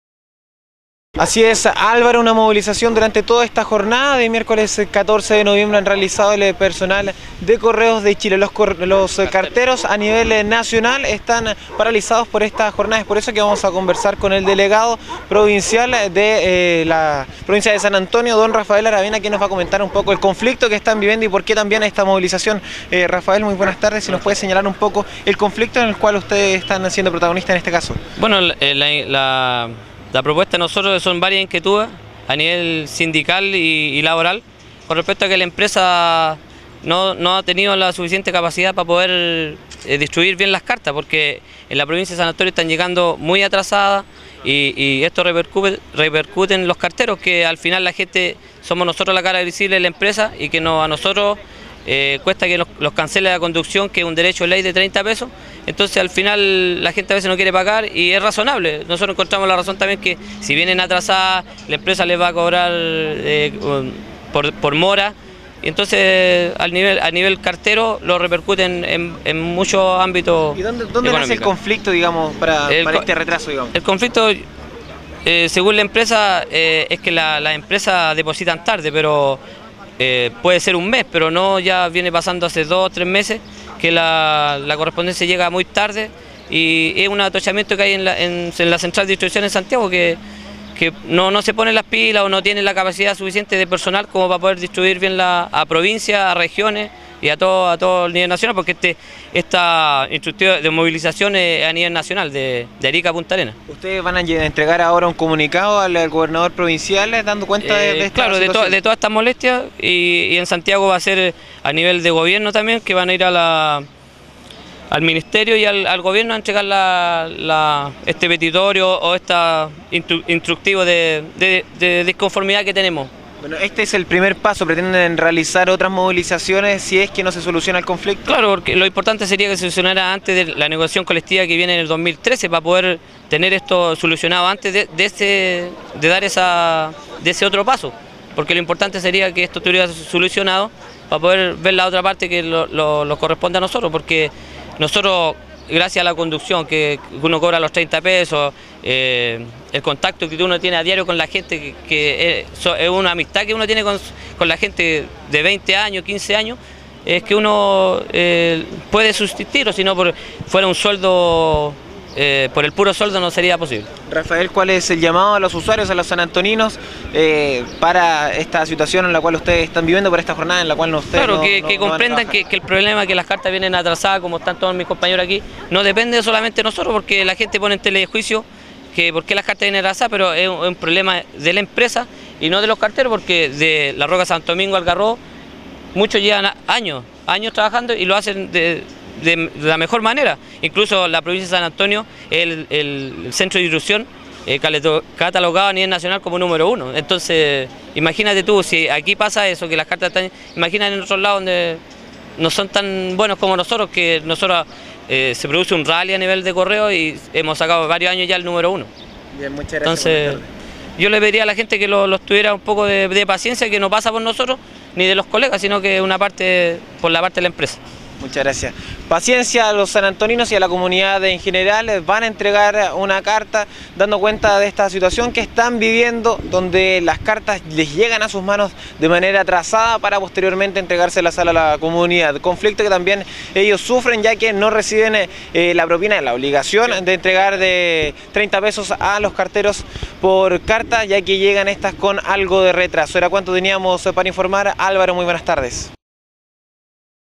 14 NOV 2012 CONTACTO CORREOS
Hoy la federación de trabajadores de correos de Chile realizó una manifestación a nivel nacional, por el retrazo de la entrega de las cartas de la central de distribución, generando una tardanza en la distribución de las cartas a los usuarios y perdiendo remuneraciones al no recibir el pago por parte de los sanantoninos, situación que documentario y entregaran al gobernador provincial. Mas detalles en el siguiente contacto con el periodista Pablo medina, adelante Pablo buenas tardes.